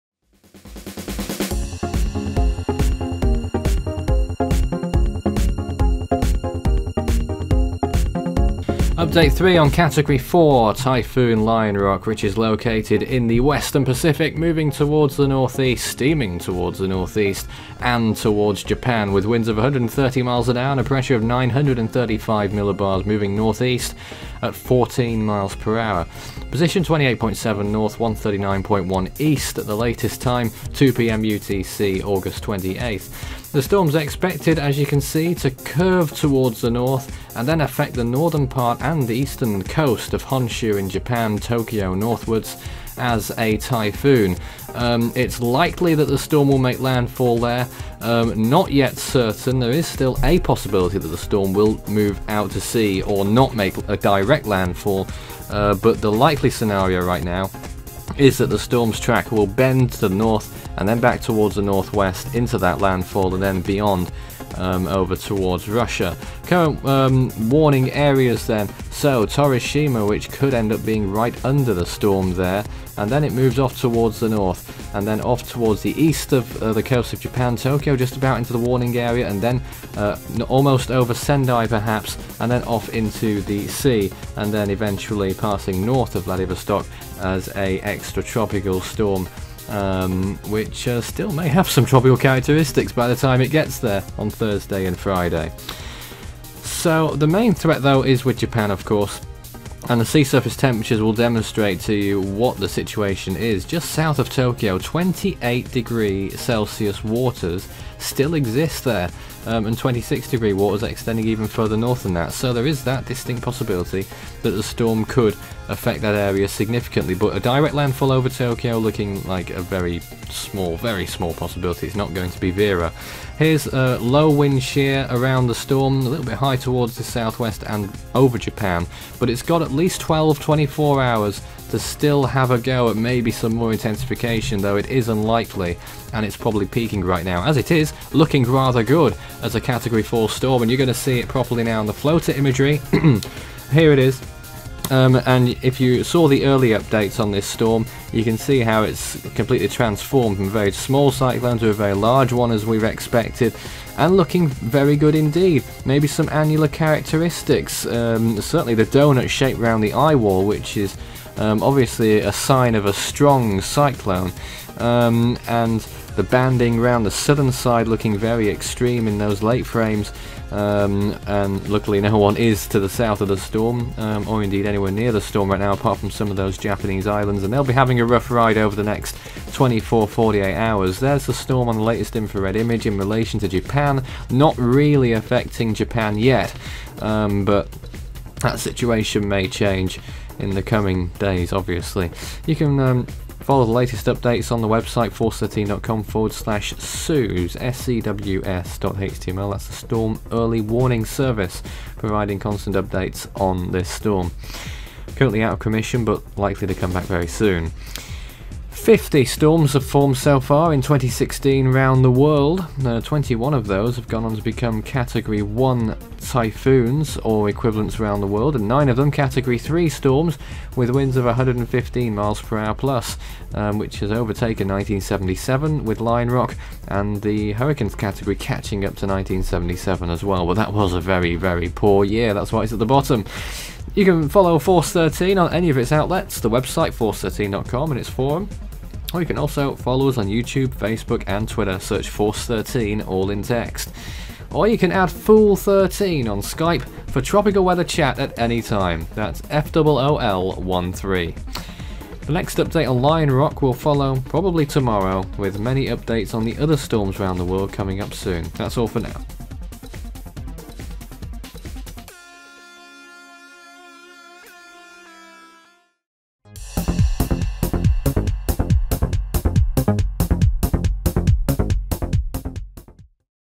Typhoon Lionrock - Update 3 (2300 JST, August 28, 2016)
Typhoon Lionrock is steaming off towards the northeast and could curve into the northern part of Japan this week.